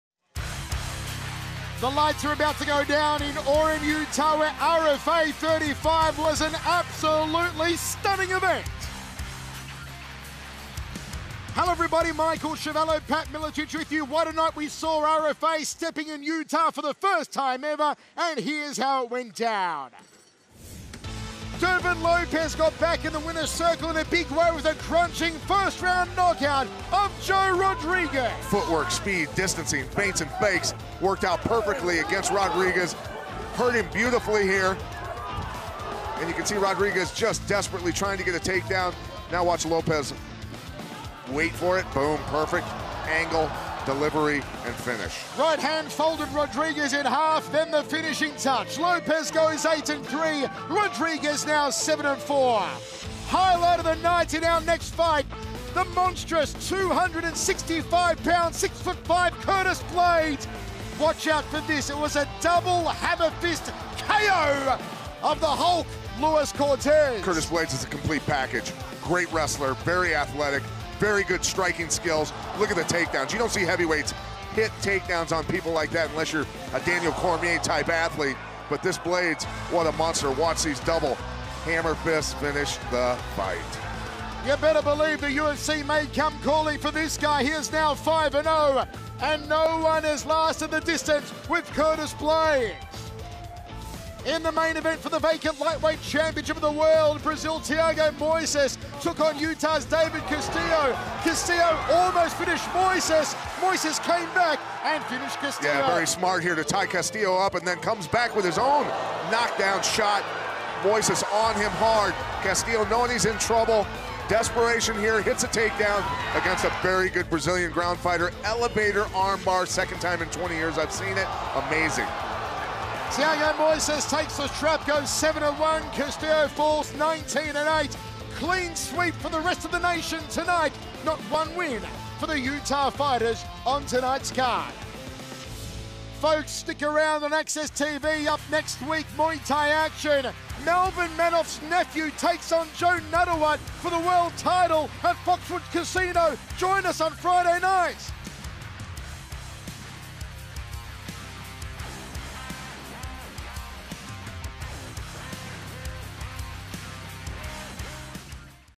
Thiago Moisés caps off a Dominant Night for the RFA against Showdown Fights | RFA 35 Highlights
Thiago Moisés is an early candidate for "Submission of the Year" with his beautiful Helicopter Armbar finish of David Castillo in the main event of RFA 35. The submission win earned him the vacant RFA lightweight title. Curtis Blaydes and Dervin also picked up brutal KO wins on Friday, February 19th in Orem, Utah.